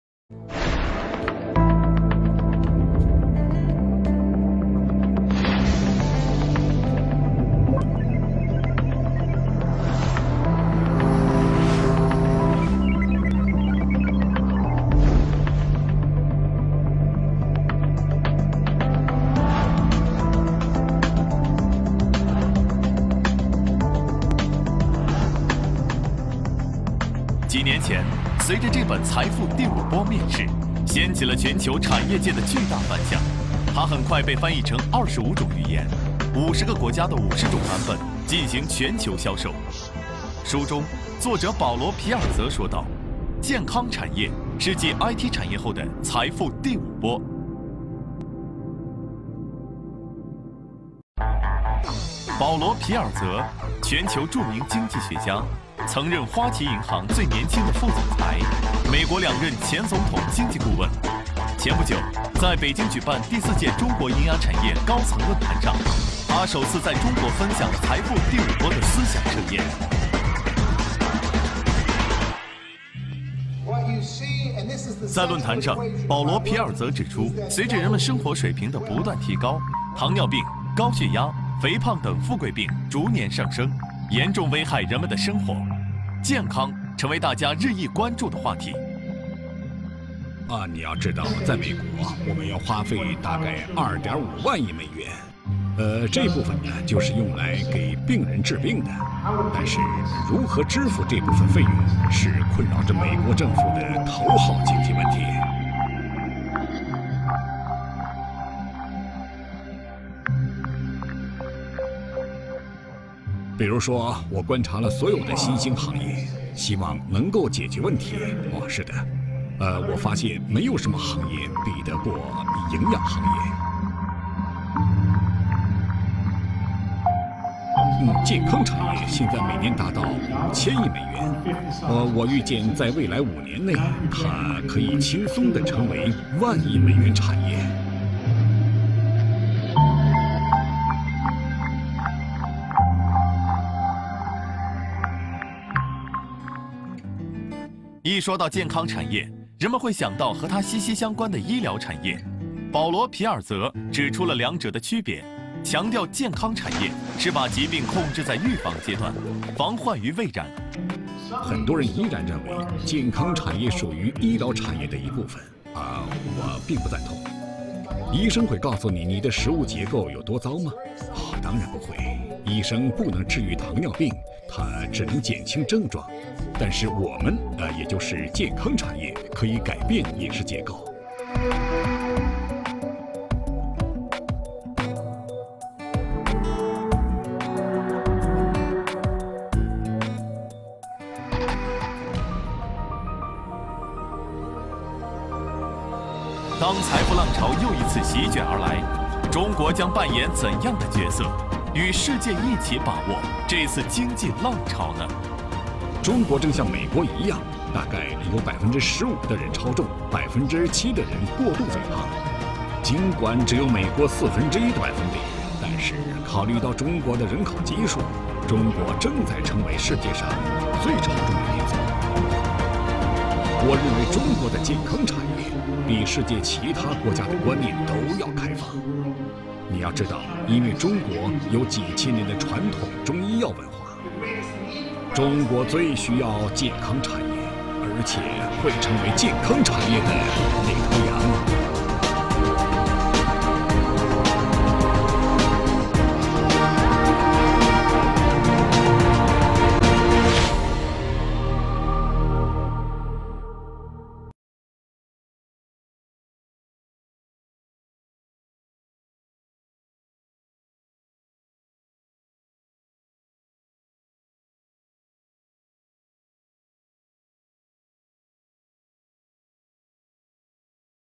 全球著名经济学家保罗·皮尔泽《财富第五波》到底是什么 ？中国将成为健康产业的领头羊
掀起全球产业界巨大反响的《财富第5波》作者，全球著名经济学家保罗·皮尔泽预言健康产业将是继IT产业后的“财富第5波”。在北京举行的第四届中国营养产业高层论坛上，他与与会者就此热点话题展开了讨论。他特别指出，中国将成为健康产业的领头羊。